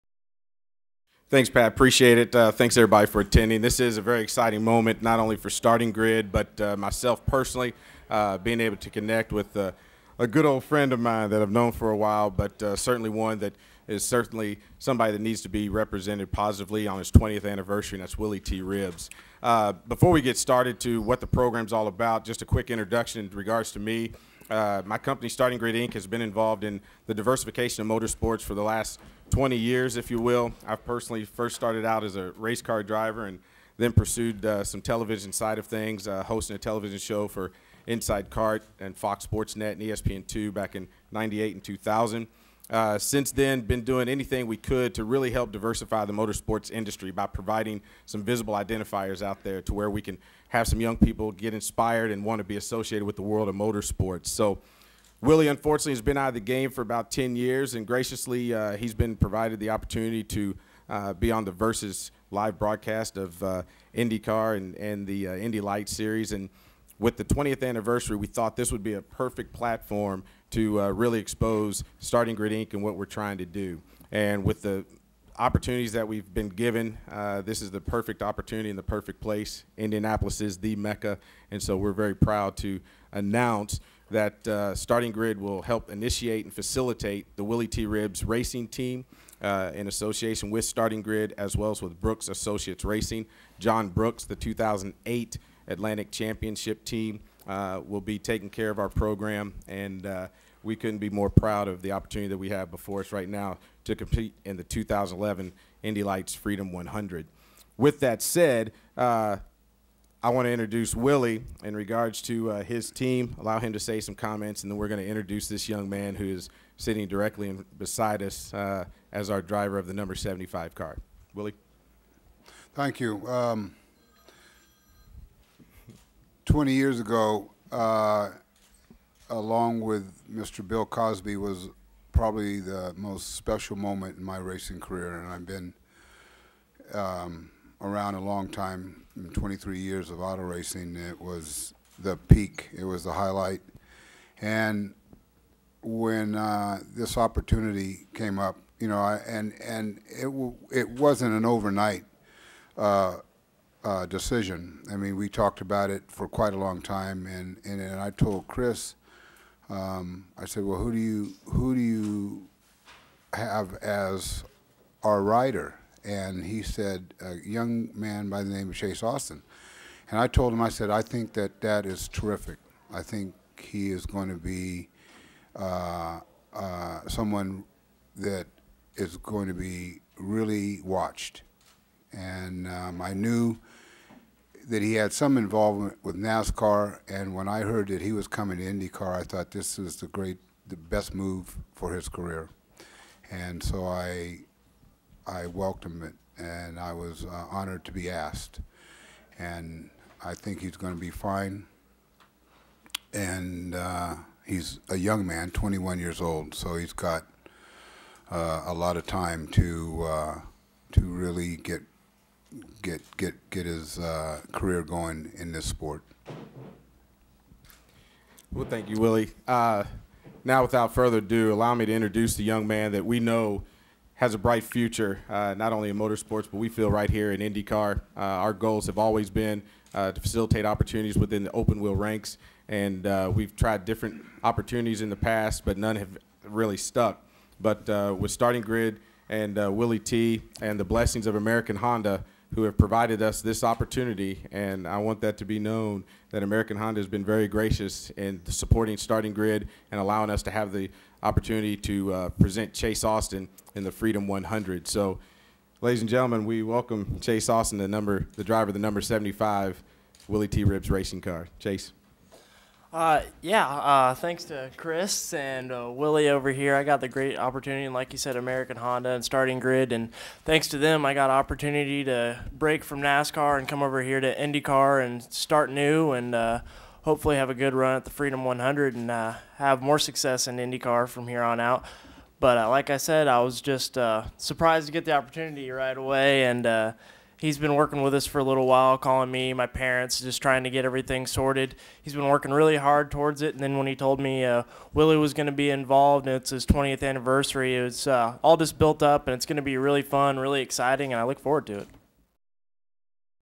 Willy T. Ribbs Press Conference
In partnership with Starting Grid, Inc. principal Chris Miles, Ribbs has formed Willy T. Ribbs Racing to campaign 21-year-old Chase Austin in the 2011 Firestone Freedom 100 at the Indianapolis Motor Speedway